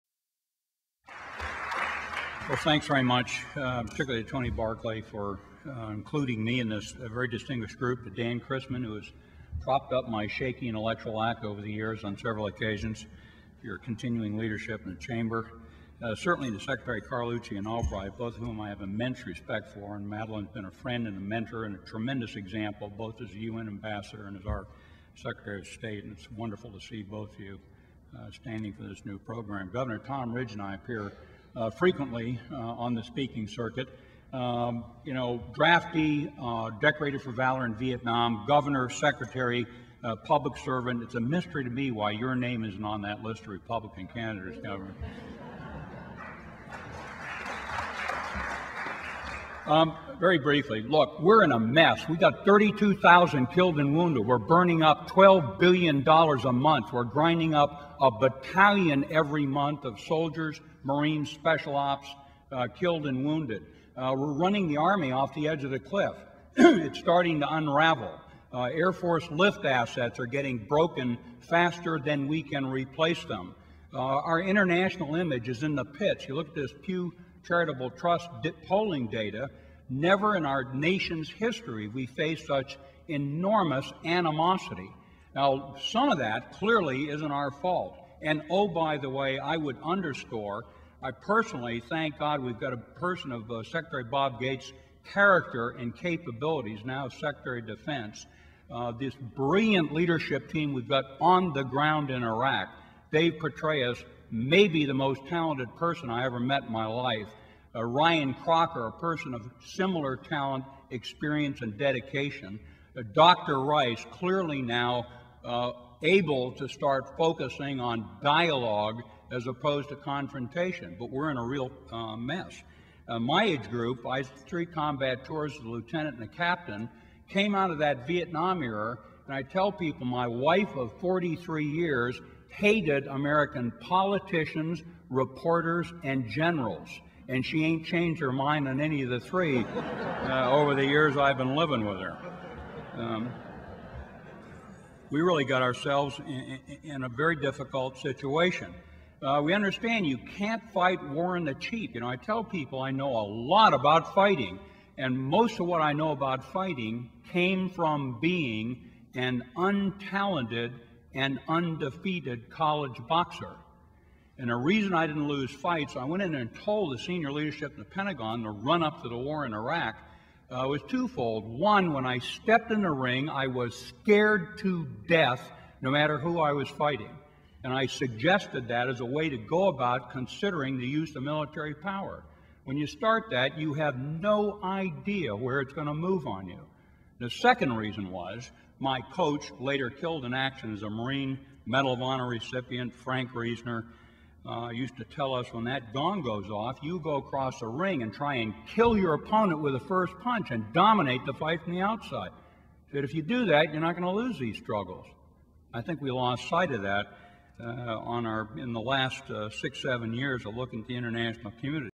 Gen. Barry McCaffrey - Center for US Global Engagement - 1
Gen. Barry McCaffrey speaking at the launch of Impact '08: Building a Better, Safer World, on July 25th, 2007. Impact '08 is the signature campaign of the Center for US Global Engagement, a non-partisan non-profit that seeks to elevate development and diplomacy in the forthcoming presidential elections. Part 1 of 2